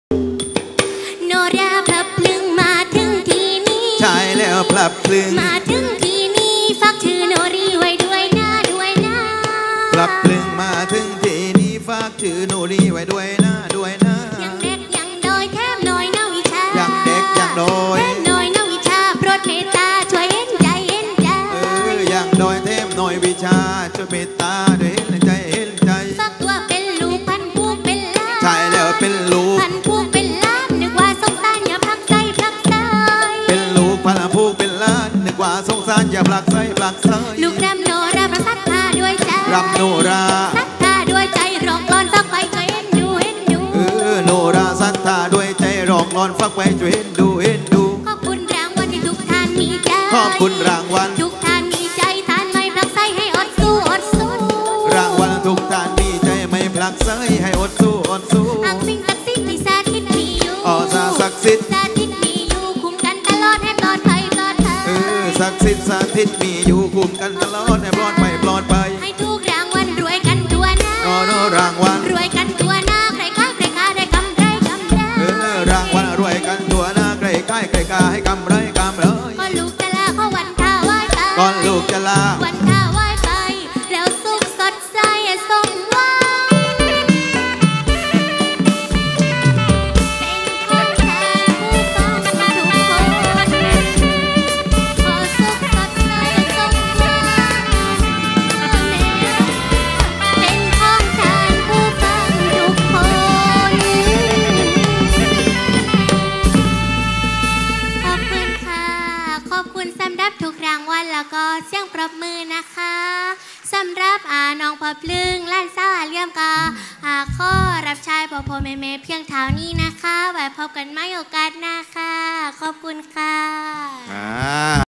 น้องพลับพลึง ว่ากลอนทอย ไข่เหลี้ยม กาชาดสตูล65
น้องพลับพลึง#ไข่เหลี้ยม กาชาดสตูล65